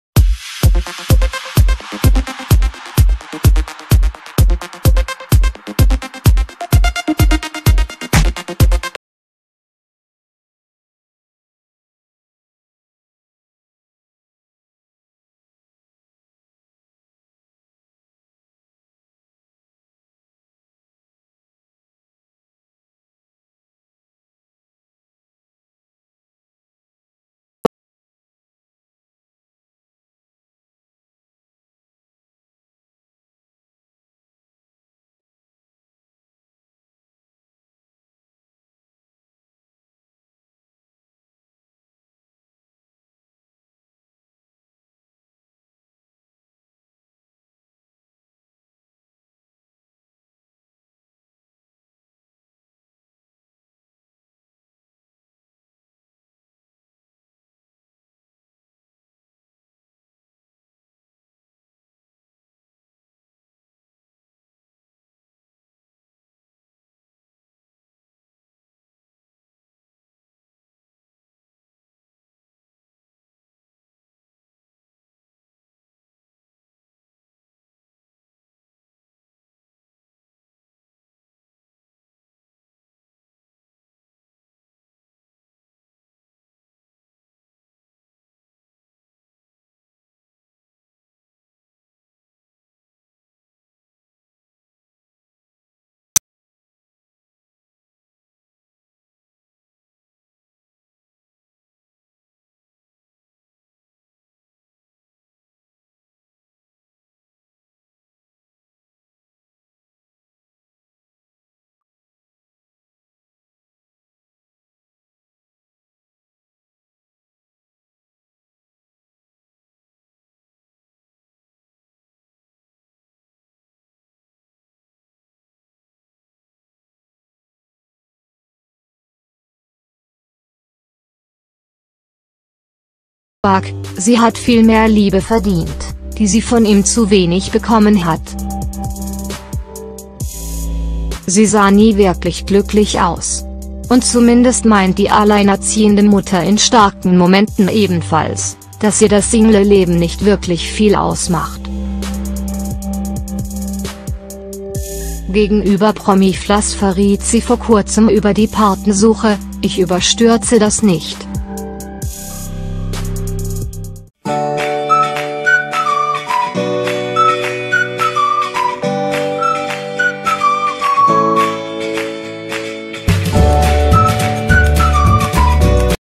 Sarah Lombardi traurig: "Wo bist du, wenn ich dich brauche?" | CELEBRITIES und GOSSIP
Wen vermisst Sarah Lombardi (25) hier so sehnsuchtsvoll? Eigentlich ist die Mama des kleinen Alessio (2) momentan voll und ganz in Karnevals-Stimmung: Kürzlich teilte sie nämlich Fotos von ihrem sexy Piratenkostüm zur Weiberfastnacht. Am Wochenende scheint sie aber nicht nur eine Erkältung, sondern auch eine melancholische Stimmung im Griff zu haben: Am Sonntag postete die Kölnerin ein Bild, auf dem sie ziemlich traurig dreinschaut!